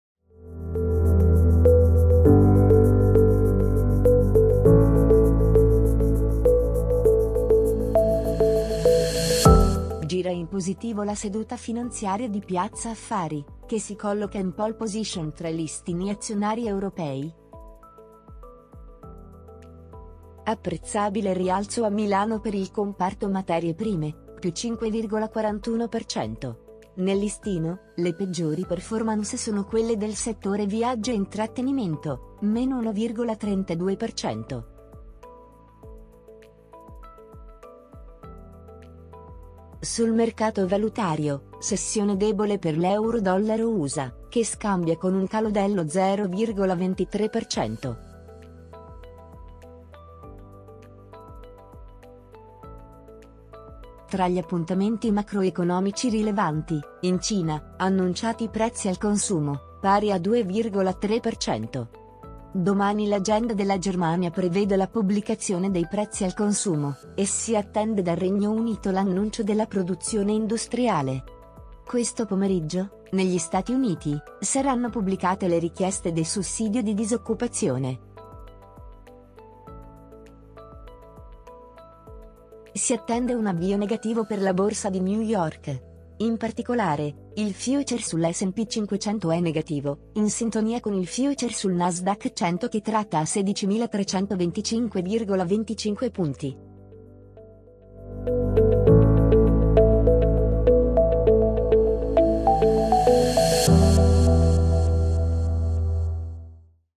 1 minuto in Borsa 9 dicembre 2021
Gira in positivo la seduta finanziaria di Piazza Affari, che si colloca in pole position tra i listini azionari europei.

Apprezzabile rialzo a Milano per il comparto materie prime (+5,41%). Nel listino, le peggiori performance sono quelle del settore viaggi e intrattenimento (-1,32%).

Sul mercato valutario, sessione debole per l'Euro / Dollaro USA, che scambia con un calo dello 0,23%.

Tra gli appuntamenti macroeconomici rilevanti, in Cina, annunciati i Prezzi al Consumo, pari a 2,3%. Domani l'agenda della Germania prevede la pubblicazione dei Prezzi al Consumo, e si attende dal Regno Unito l'annuncio della Produzione industriale. Questo pomeriggio, negli Stati Uniti, saranno pubblicate le Richieste dei Sussidi di Disoccupazione.

Si attende un avvio negativo per la Borsa di New York. In particolare, il future sull'S&P 500 è negativo, in sintonia con il future sul Nasdaq 100 che tratta a 16.325,25 punti.

Sui social